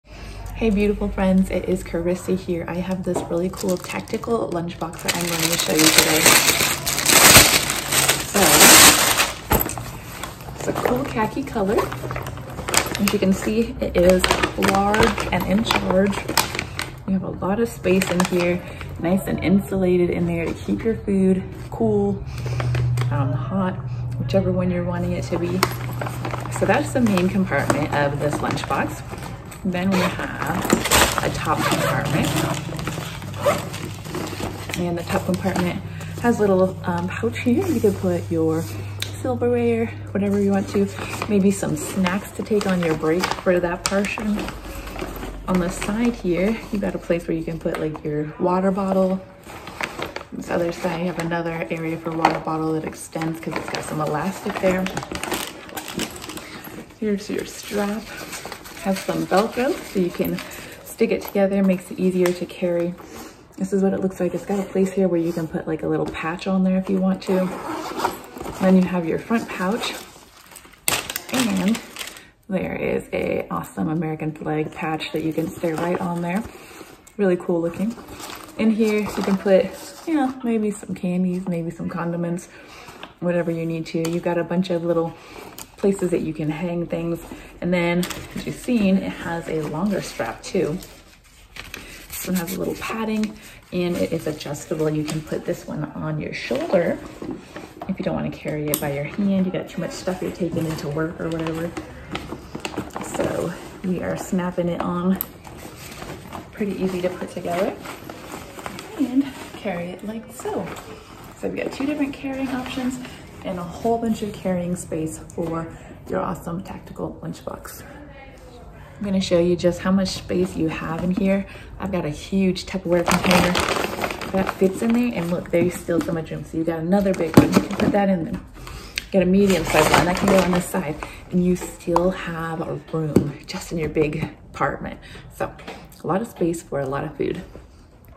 I didn't know they made lunch boxes this big or with this many cool features
I eat a lot and need this big amount of space to carry it all.